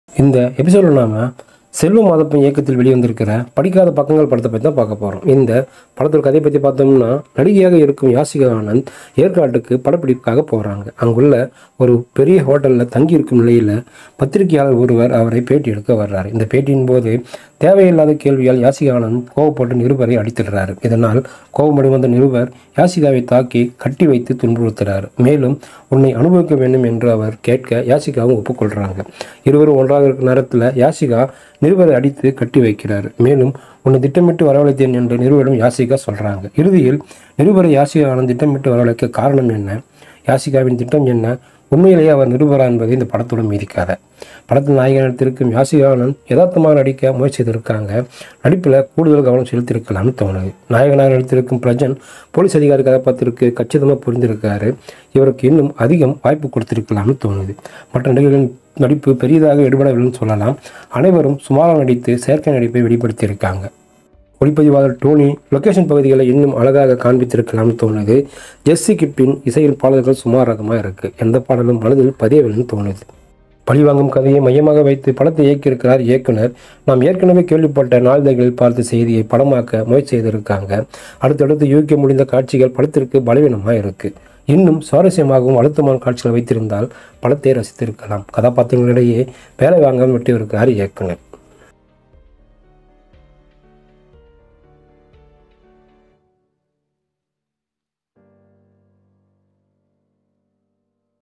Padikkadha Pakkangal - Review | Yashika Aannand | Prajin | Selvam
I have given a review about this film and this is my personal opinion and I am not referring to anyone